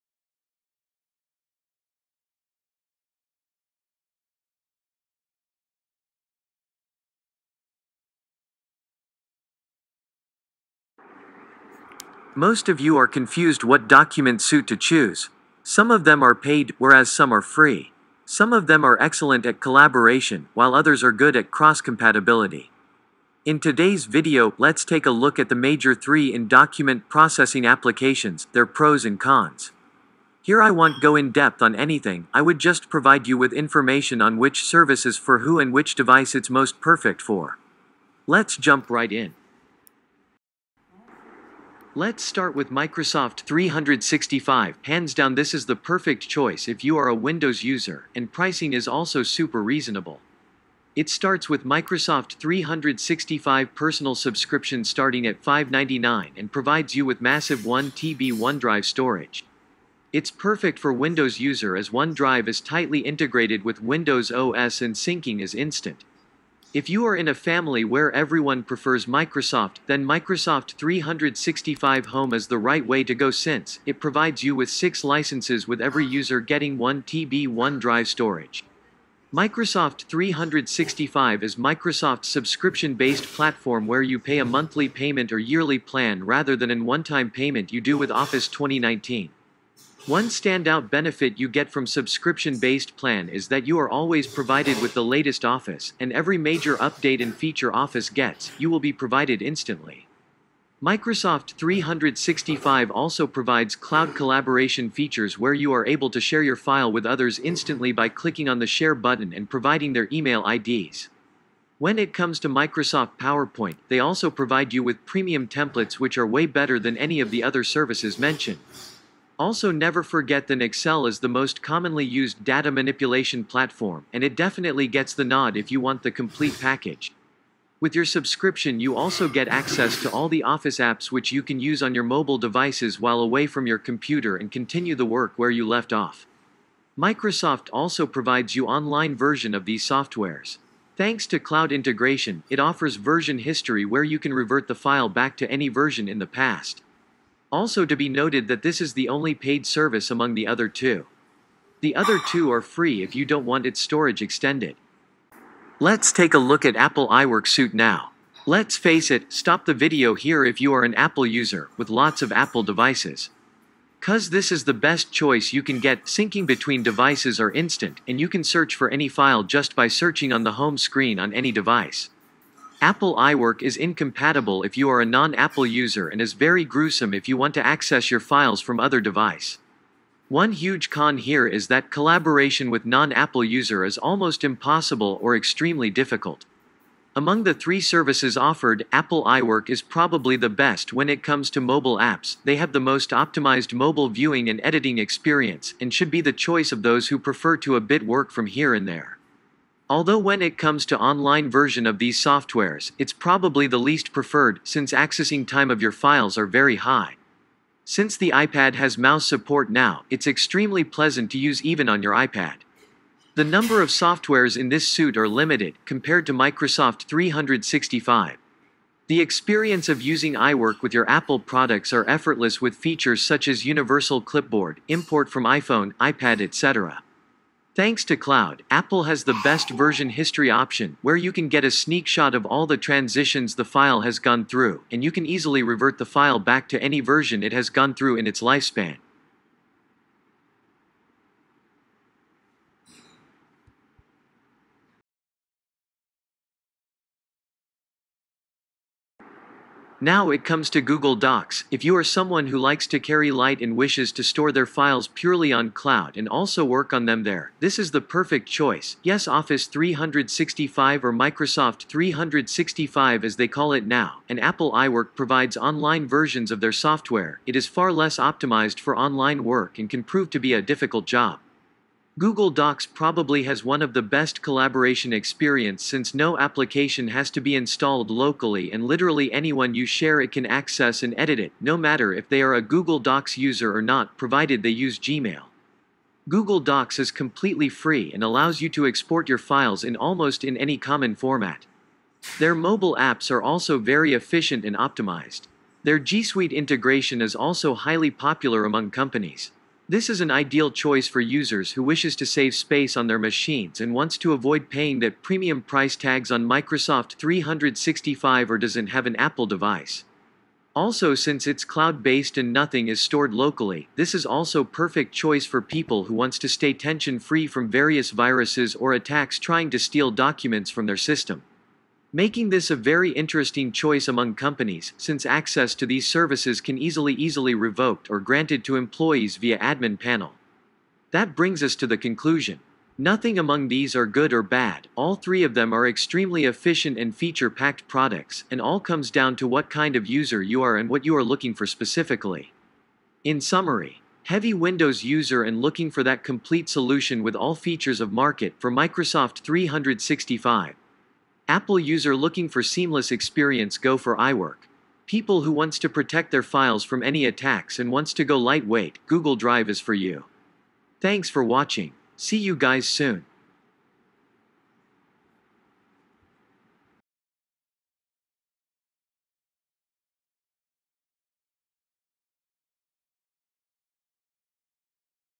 MICROSOFT 365 vs APPLE iWORK vs GOOGLE DOCS
Let's compare the top 3 document processing suit and decide which is the best, or which is for who.